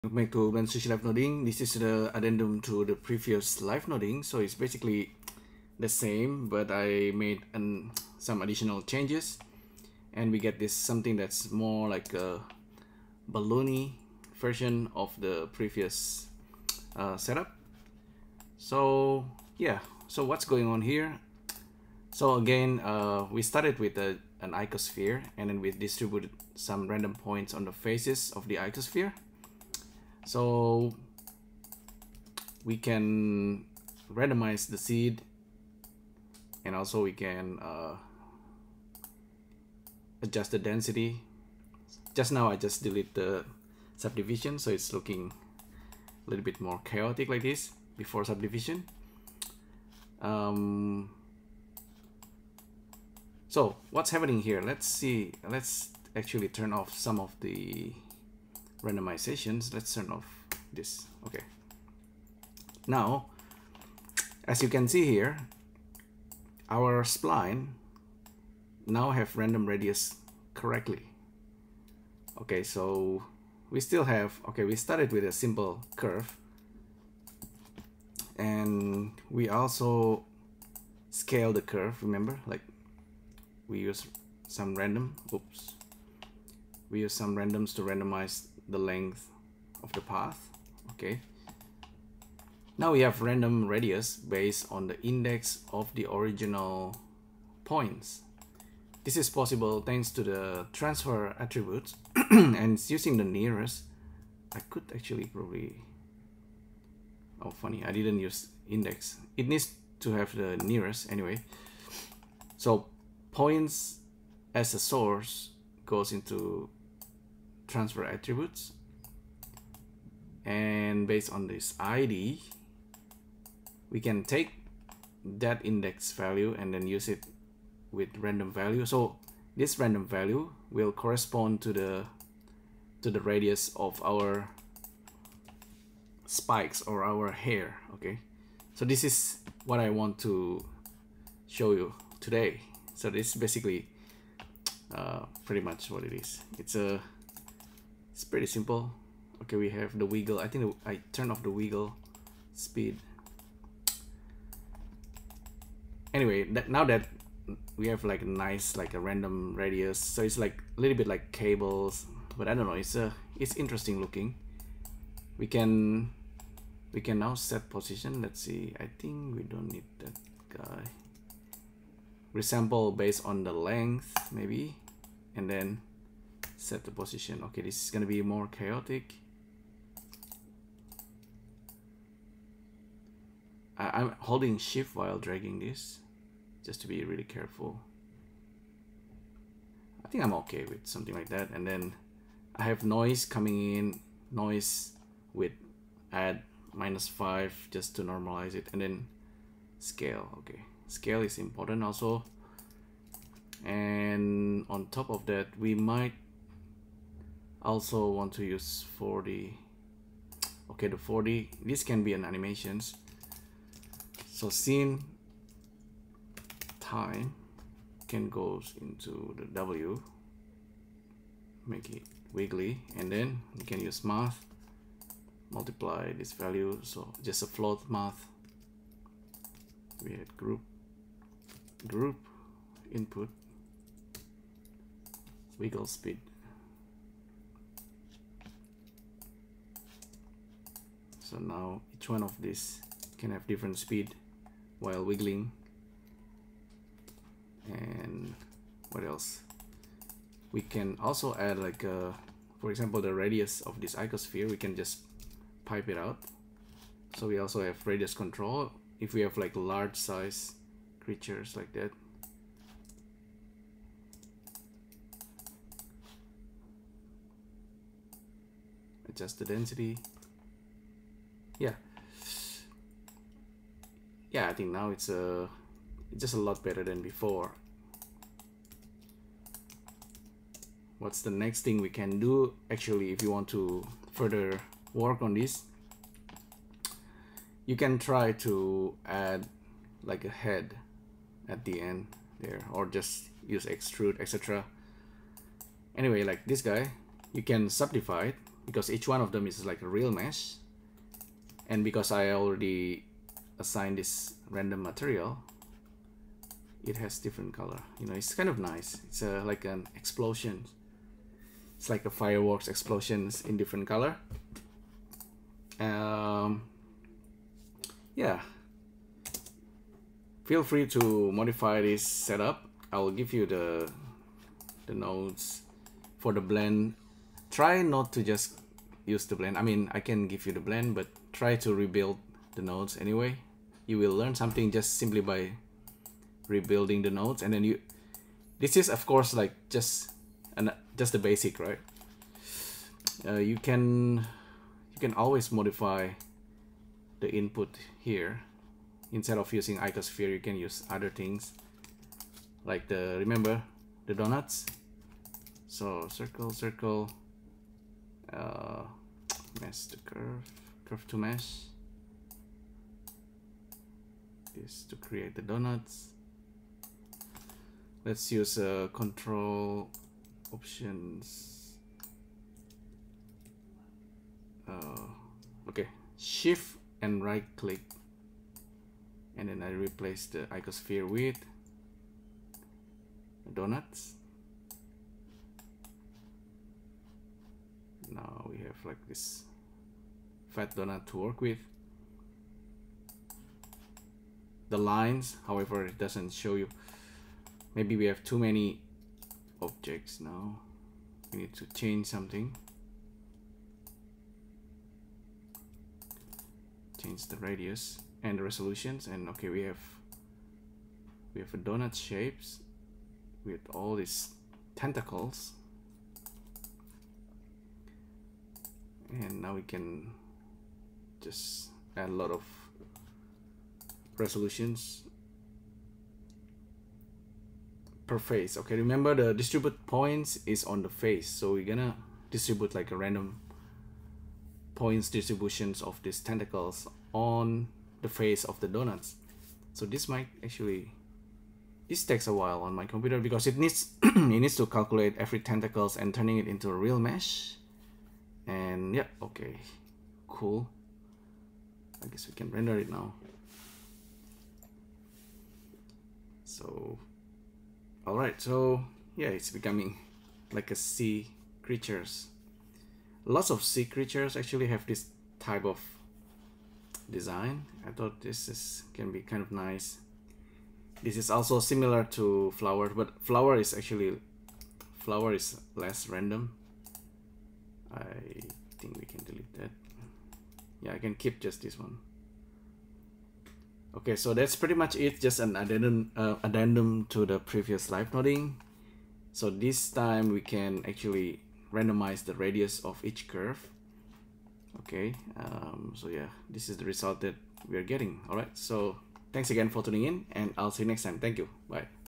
LIVENODING Chaotic Tentacles Hair Using Geometry Nodes
This is an addendum to the previous LIVENODING node setup we created using Geometry Nodes.

More controls added:
- Random Radius based on ID of the original points, thanks to Transfer Attribute node. Cheers to Brady:
- Wiggle speed adjusted

NOTE: You can try also use information of curve parameter to adjust the radius along path.